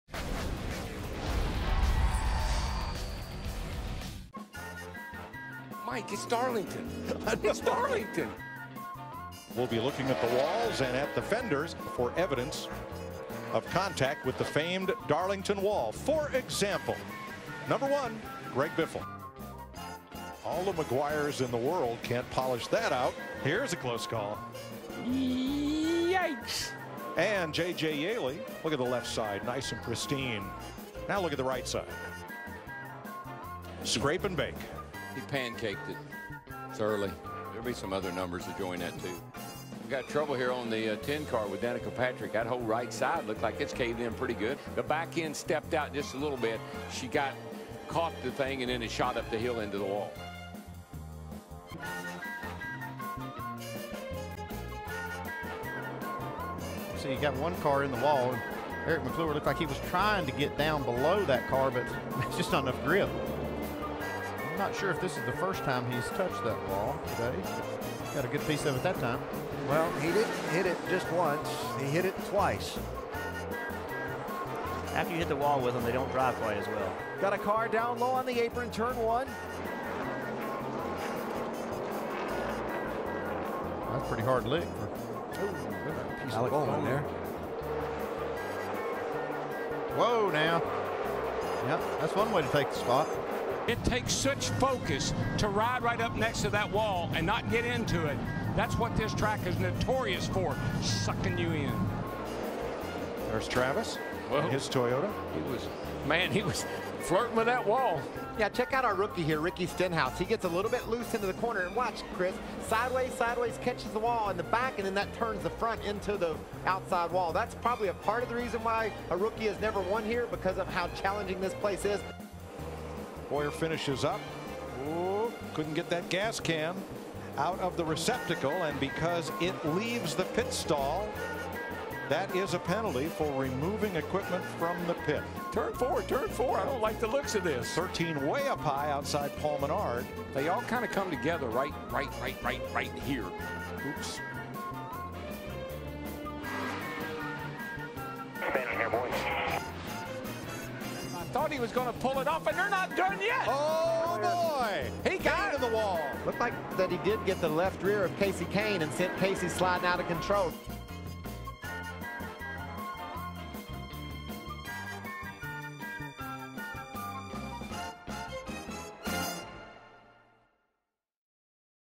Darlington's Greatest Hits
Highlights of drivers earning their Darlington stripes!